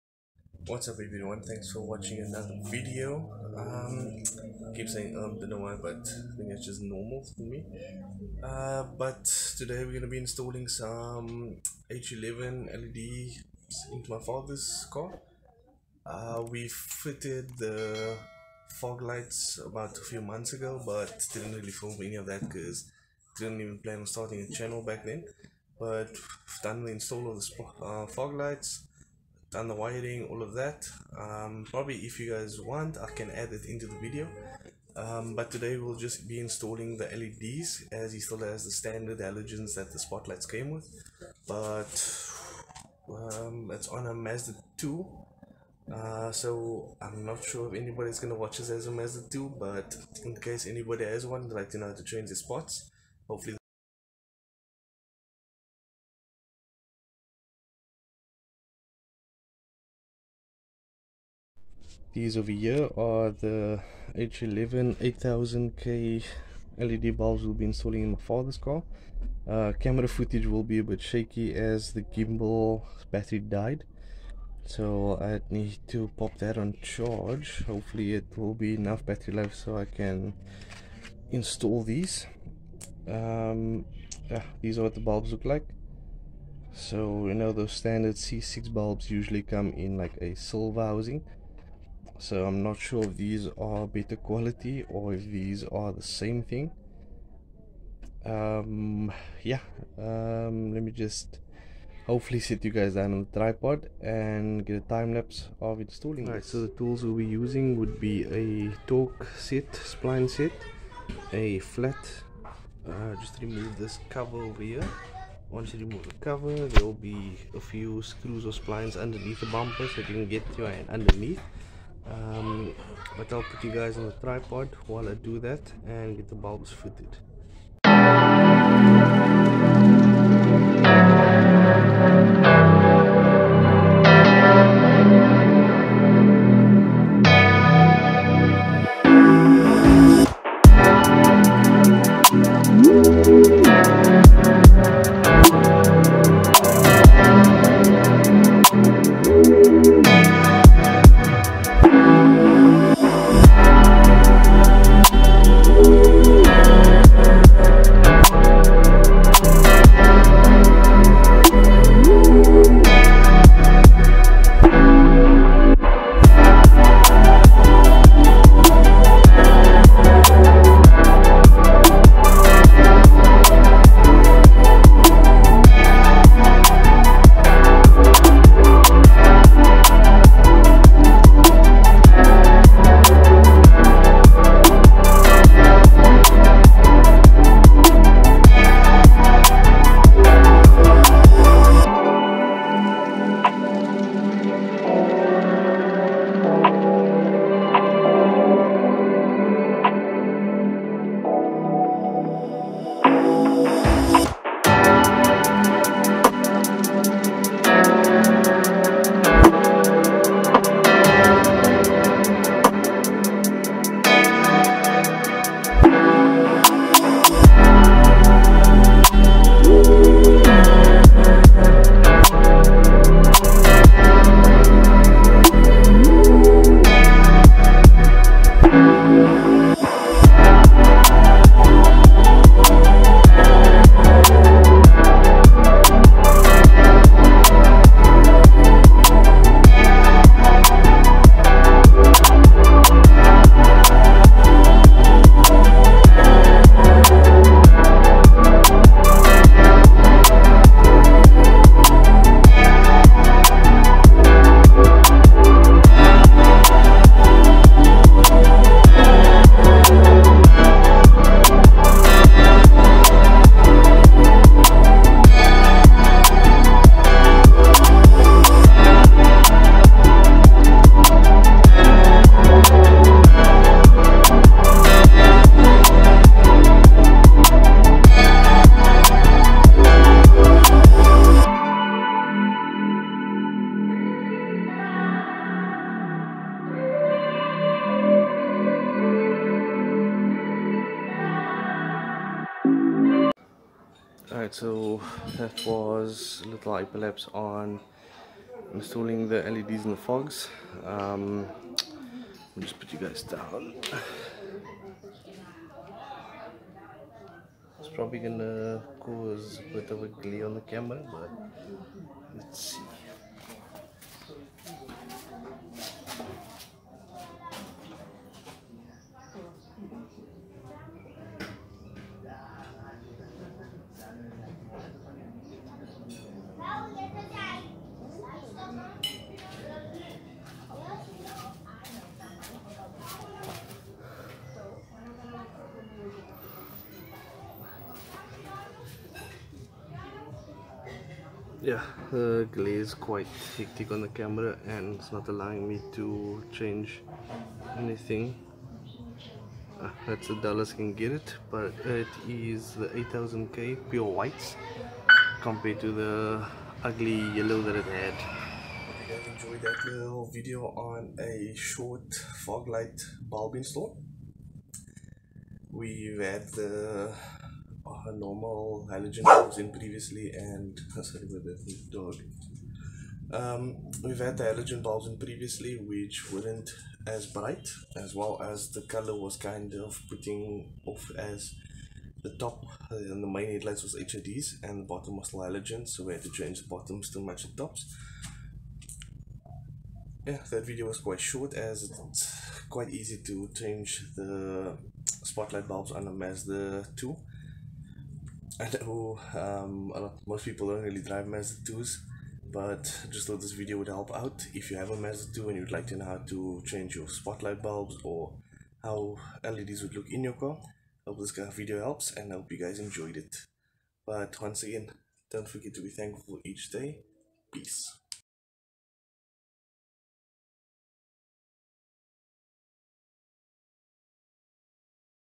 MAZDA 2 LED FOGLIGHT BULB INSTALL
In today's video we'll be installing a set of 8000k H11 LED Bulbs in a Mazda 2 sedan.